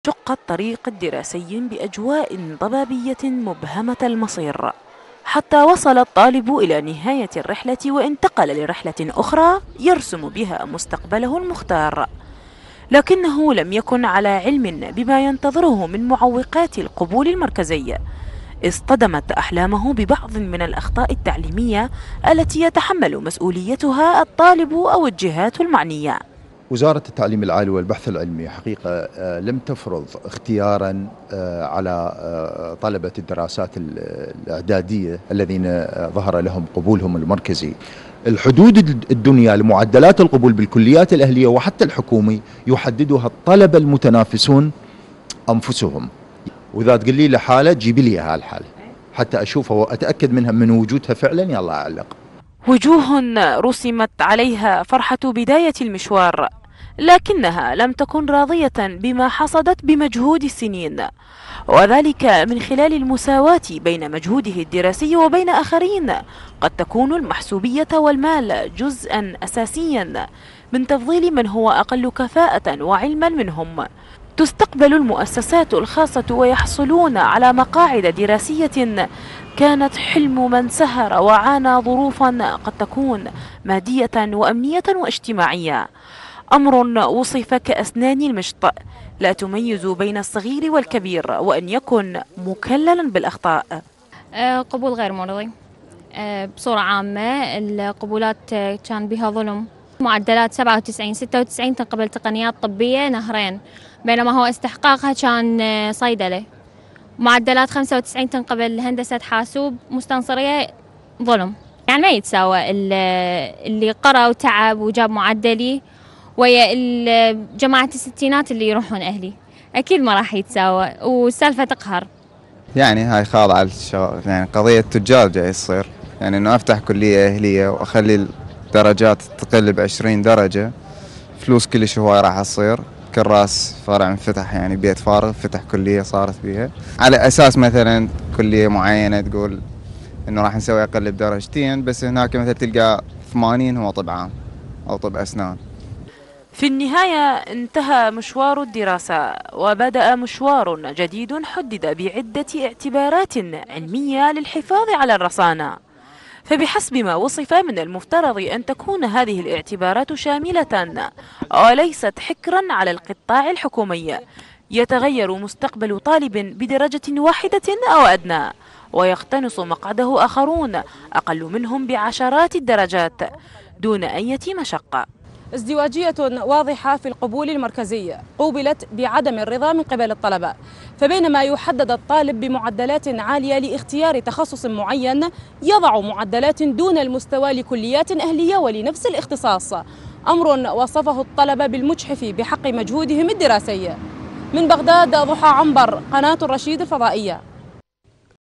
نتائج القبول المركزي.. بين فرحة الاستقبال وخيبة امل الآخرين
فرحة عمت بعض الطلبة باستقبالهم بداية العام الدراسي للجامعات، فيما اصطدم آخرون بنتائج القبول المركزي التي حطمت كل طموحاتهم واحلامهم فضلا عن عدم مساواة المقاعد الدراسية بينهم..